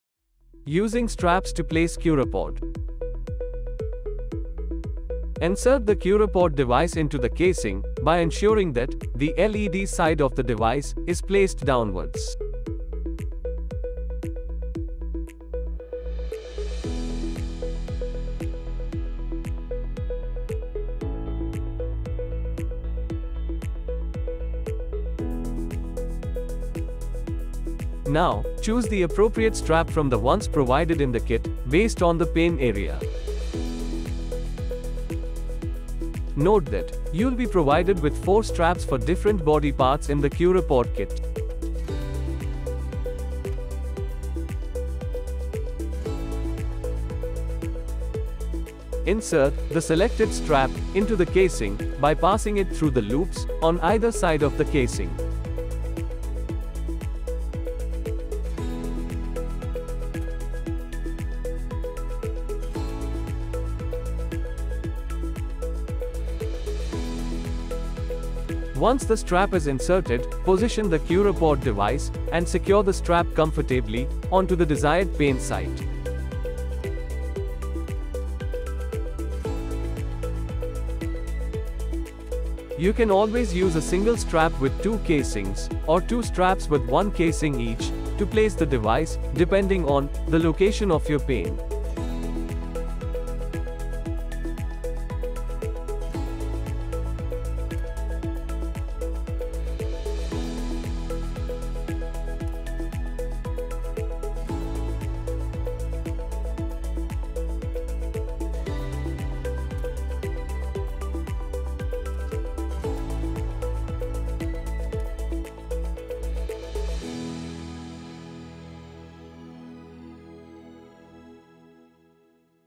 Using Curapod Straps: A Simple Guide
Curapod straps are designed to enhance the effectiveness and convenience of using your Curapod device. Here's a step-by-step guide on how to use them:
1. Place the Curapod in the casing by the LED side downside
2. Choose appropriate straps as you are provided with 4 straps
3. Insert the strap into the devices
4. Position and wrap the strap around the designated body part and fasten it securely.
Now, switch on your Curapod and set the desired settings using the control panel or the accompanying mobile app. Sit back, relax, and let Curapod do its magic.
Refer to the user manual or consult with a healthcare professional for any specific instructions or guidelines regarding the use of Curapod straps for your individual needs.